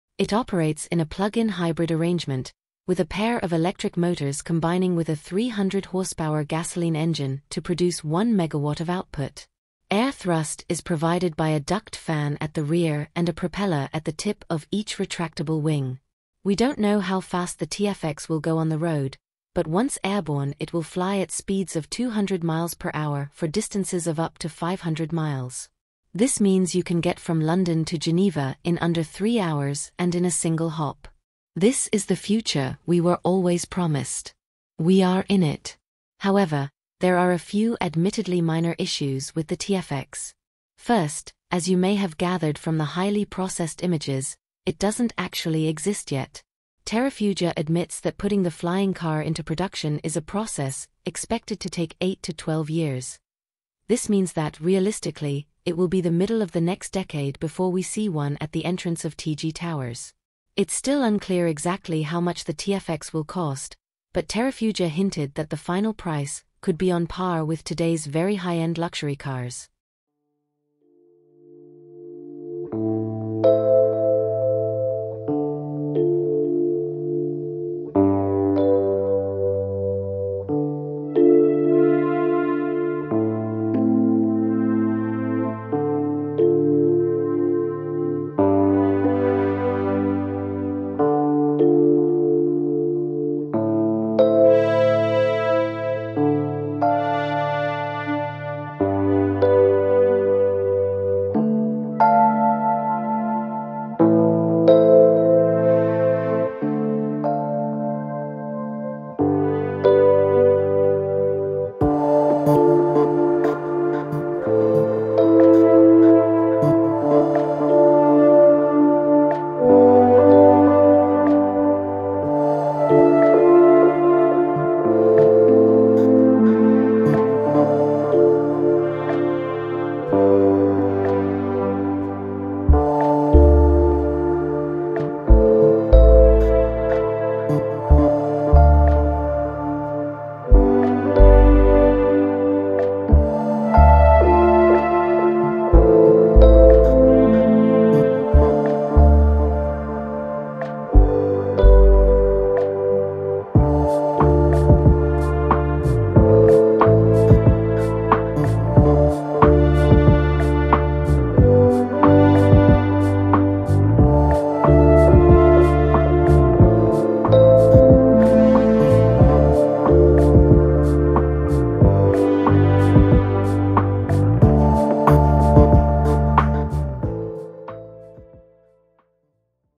Will Soar for Distances of 500 miles | Can It Be Real? | Terrafugia TF-X Flying Car Concept
It operates in a plug-in hybrid arrangement, with a pair of electric motors combining with a 300-horsepower gasoline engine to produce one megawatt of output. Air thrust is provided by a ducted fan at the rear and a propeller at the tip of each retractable wing.

We don't know how fast the TF-X will go on the road, but once airborne it will fly at speeds of 200mph for distances of up to 500 miles. This means you can get from London to Geneva in under three hours and in a single hop. This is the future we were always promised. We are in it.

However, there are a few admittedly minor issues with the TF-X. First, as you may have gathered from the highly processed images, it doesn't actually exist yet.

Terrafugia admits that putting the flying car into production is a process 'expected to take 8-12 years'; This means that, realistically, it will be the middle of the next decade before we see one at the entrance of TG Towers.

It's still unclear exactly how much the TF-X will cost, but Terrafugia hinted that the final price 'could be on par with today's very high-end luxury cars'.